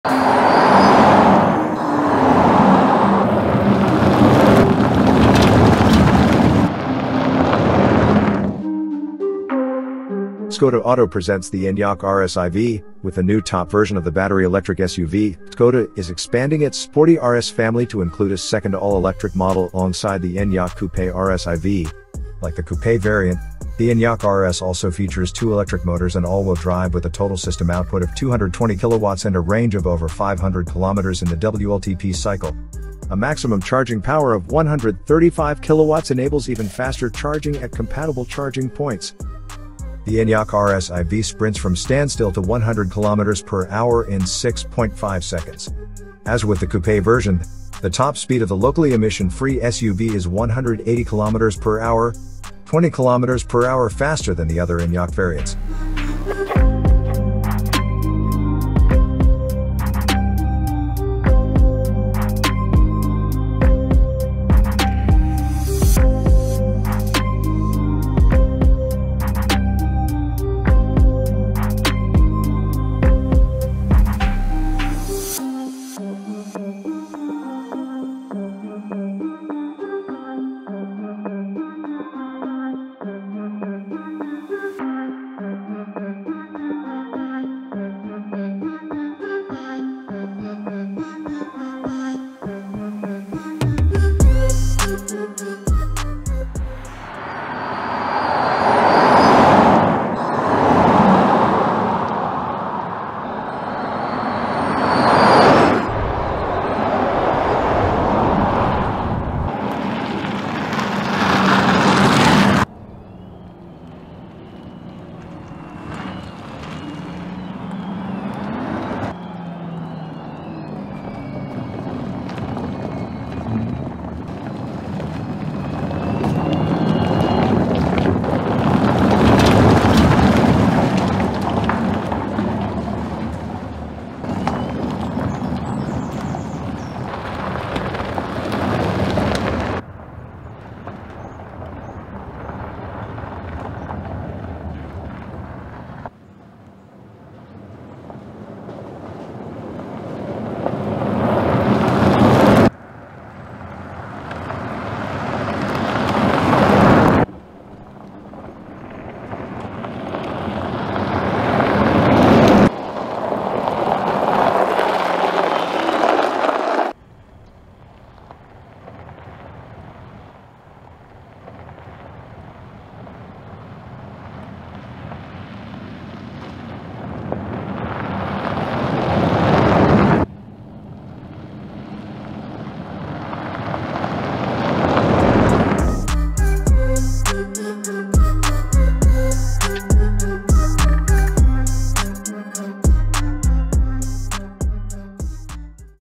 New 2023 Skoda ENYAQ RS iV | Mamba Green | Electric Sound, Trail Driving, Exterior & Interior
Take a look in this video at the new 2023 Skoda ENYAQ RS iV in Mamba Green color.

You'll see in detail:
0:00 Intro
0:10 Exterior
0:37 Interior
0:50 Trunk
1:01 Road driving
2:17 Trail driving

The ENYAQ RS iV has all-wheel drive with two electric motors, one on the front and one on the rear axle. The system output is 220 kW, and the maximum torque of 460 Nm is available immediately, as is typical of an electric vehicle. This allows the ENYAQ RS iV to sprint from standstill to 100 km/h in 6.5 seconds. As with the coupé version, the top speed of the locally
emission-free SUV is 180 km/h, 20 km/h faster than the other ENYAQ iV variants. The lithium-ion high-voltage battery with a capacity of 82 kWh (net 77 kWh) delivers a range of over 500 kilometres in the WLTP cycle. The ENYAQ RS iV can also tow a trailer weighing up to 1,400 kilograms on gradients of up to eight per cent.

Source : Skoda